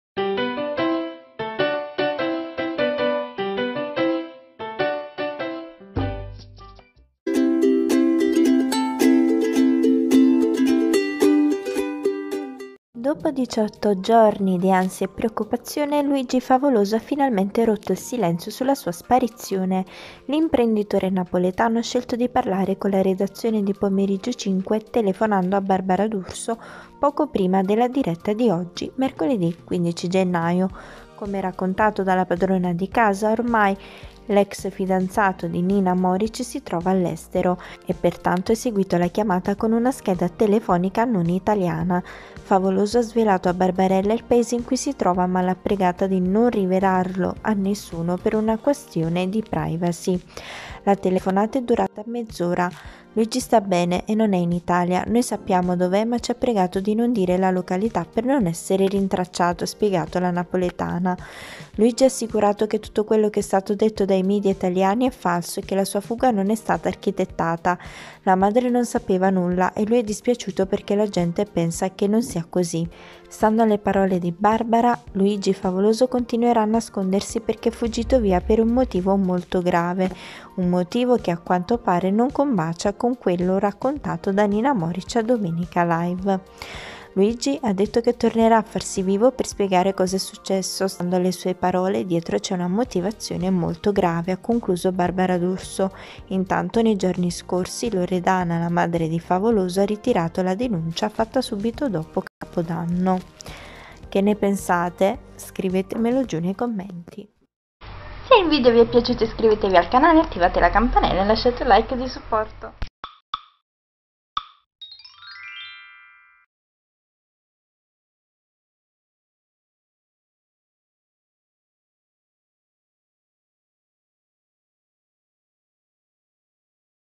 Luigi Favoloso telefona a Barbara D'Urso: è scappato per un grave motivo
Luigi Favoloso telefona alla D'Urso.